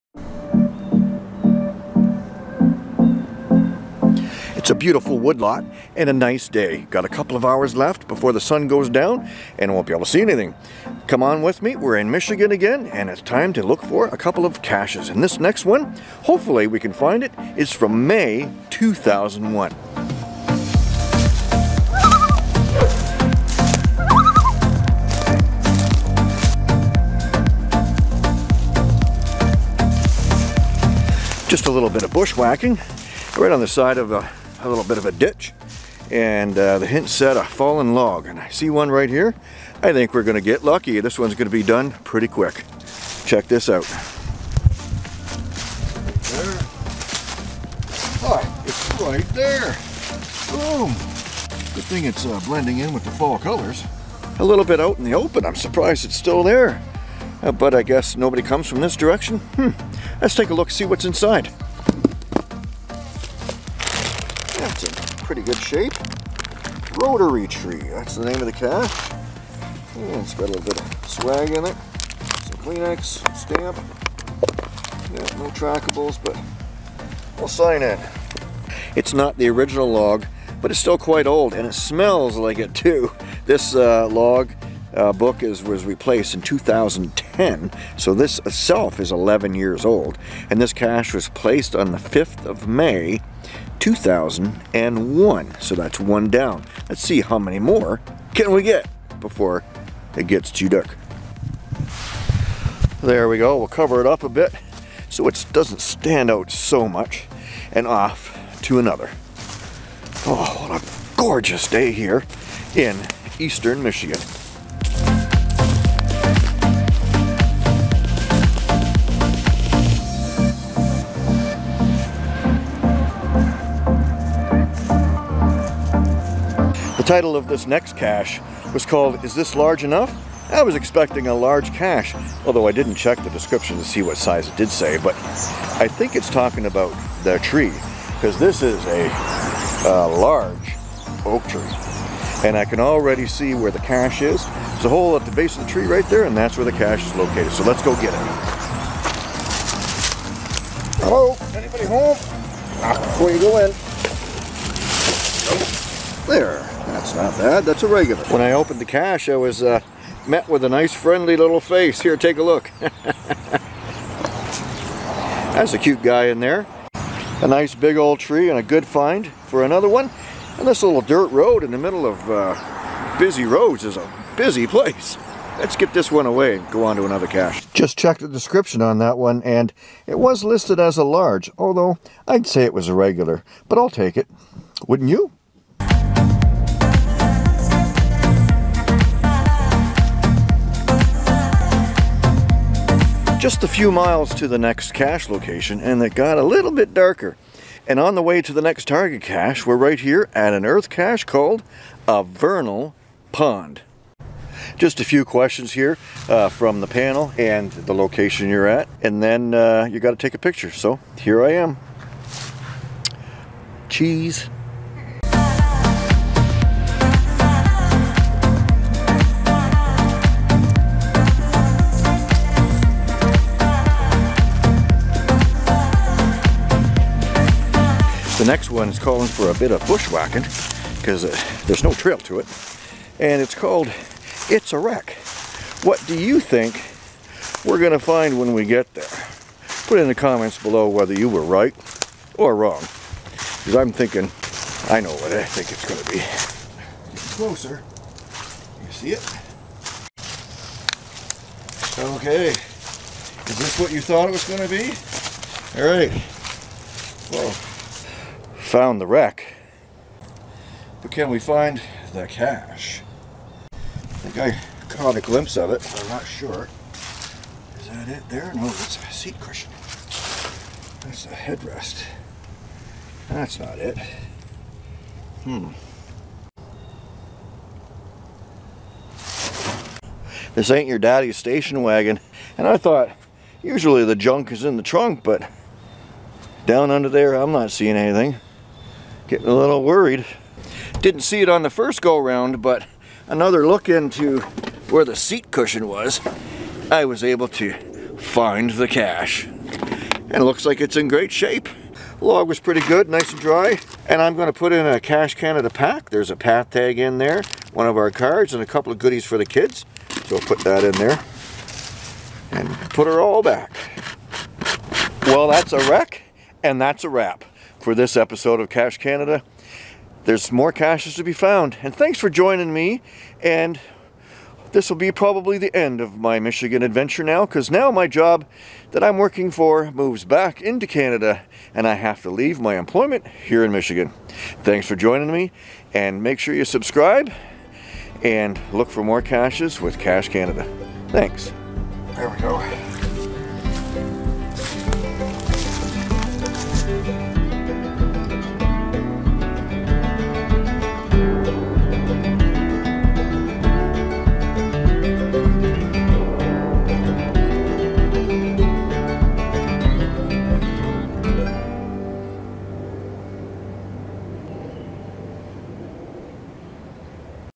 More and more Michigan
Michigan caching again as londonwesty searches for some specific caches near where he was working. Come see what treasures were in store.